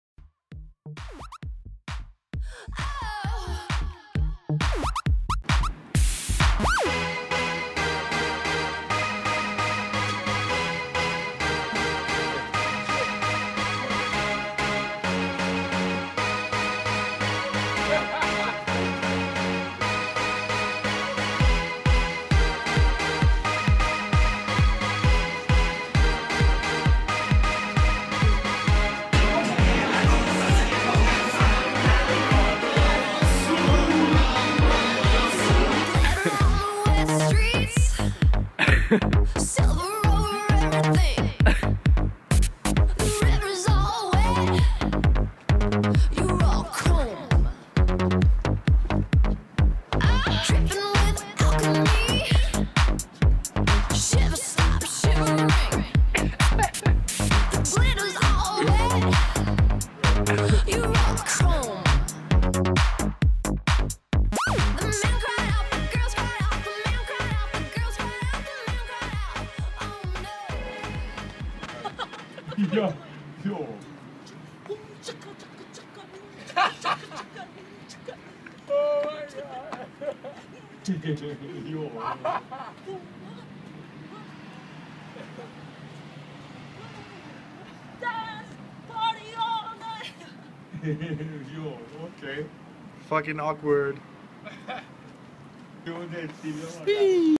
Budapest Fools 2013
Our amazing time in Budapest, 2013. 3 idiots with a camera waiting for the tram.